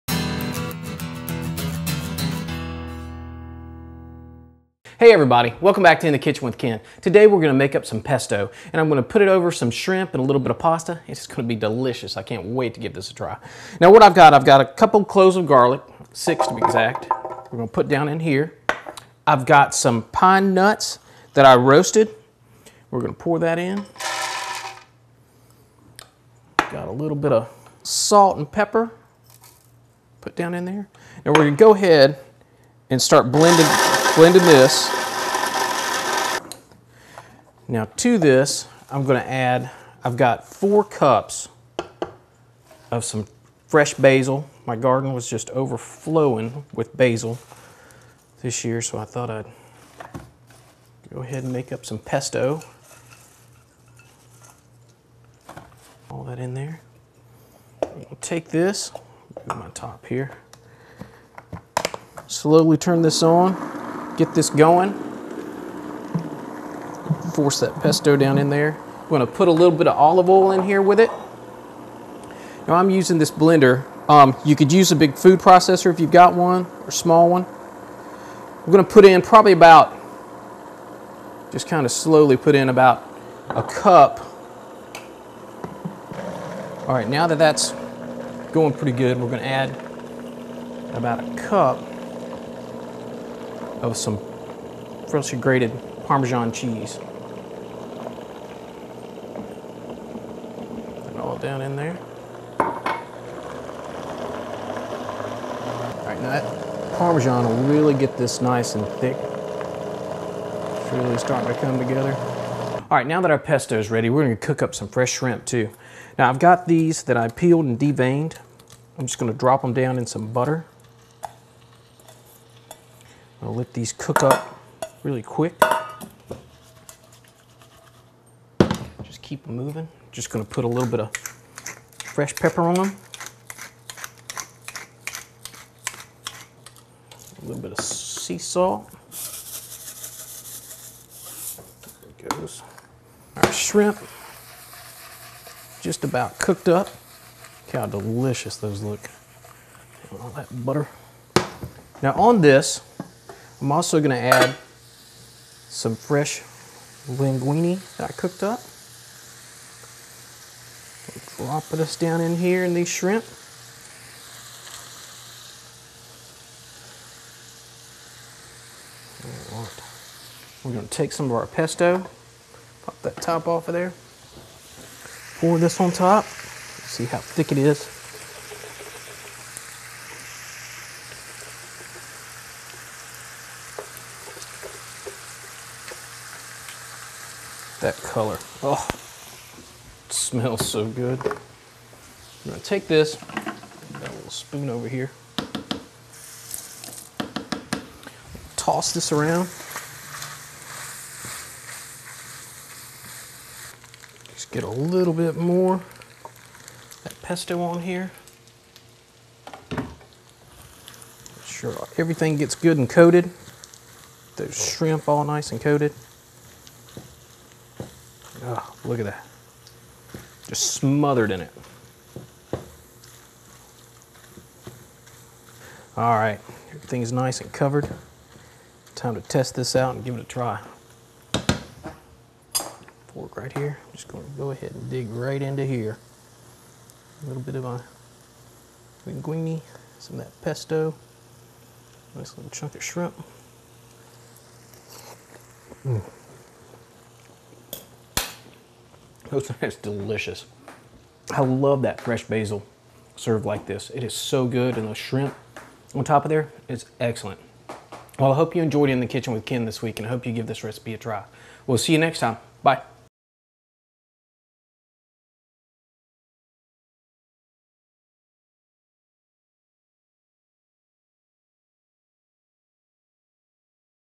In the Kitchen with Ken: Pesto, Pasta and Shrimp
Pesto, pasta and shrimp

6 garlic cloves

½ cup toasted pine nuts

½ tablespoon sea salt

1 tablespoon ground black pepper

4 cups basil leaves

1 cup grated Parmesan cheese

1 cup extra virgin olive oil

1 pound medium-sized shrimp

¼ stick of butter

Linguine pasta

For the pesto

In a blender or food processor, add garlic, pine nuts, salt and pepper. Blend until everything is chopped up nicely. Add basil leaves and begin processing slowly. You might have to push some of the leaves down inside the mixture. The, slowly add olive oil while blending. Once olive oil has been incorporated, add Parmesan cheese and mix until smooth.

For the shrimp and pasta

Cook linguine until al dente in large pot of boiling, salted water.

Meanwhile, peel and devein shrimp. Saute in hot skillet with ¼ stick of butter and cook until done. Add the cooked linguine pasta and toss with pesto. Serve hot.